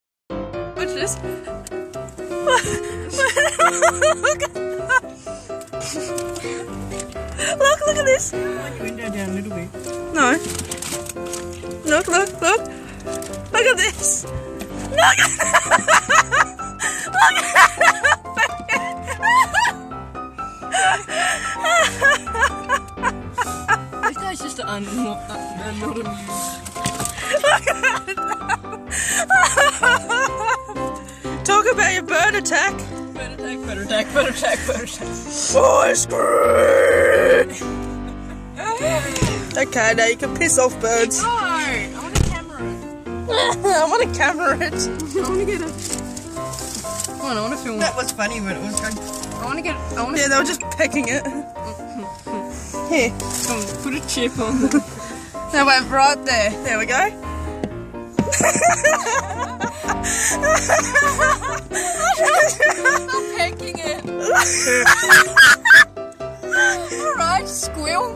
SEAGULL ATTACK
Mum tiarna and I were eating chips and Montrose bay when we started feeding the seagulls and got overun by them